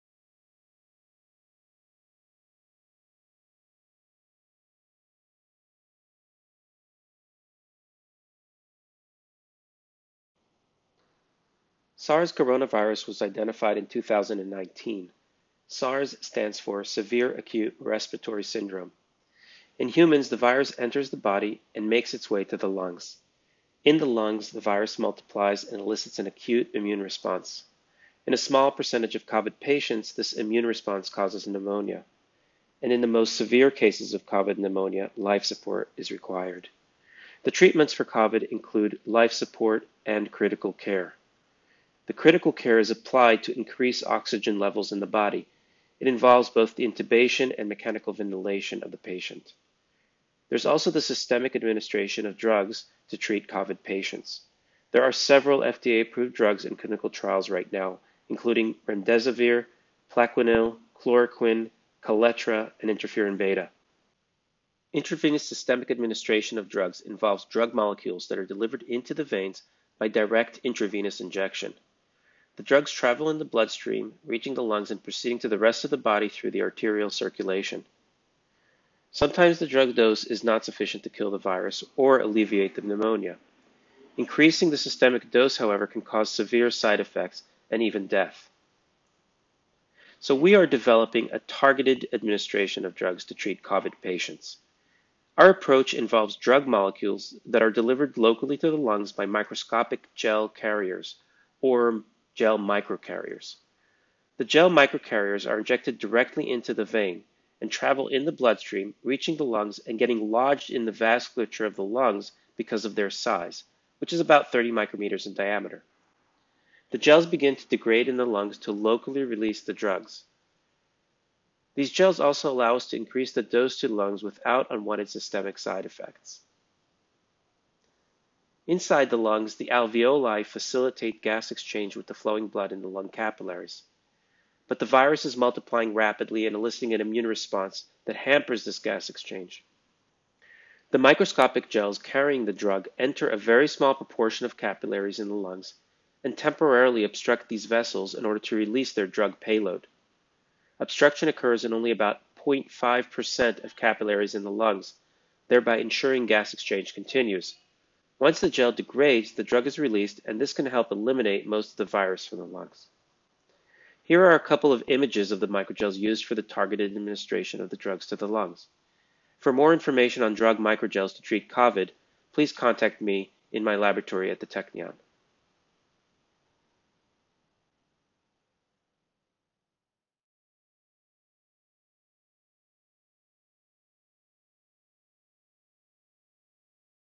Fighting Covid-19 Drug Encapsulation in Microgels Delivery to Lungs Prof. Dror Seliktar Technion
Prof. Dror Seliktar of Technion-Israel Institute of Technology's Faculty of Biomedical Engineering and his research team are currently developing an innovative technology, the  targeted administration of drugs to treat Covid-19 (Coronavirus) patients.
Prof. Seliktar explains how this works. Drug-laden Microgels can be utilized for  localized delivery into the lungs by intravenous injection. This can provide  efficient treatment of critically ill coronavirus patients.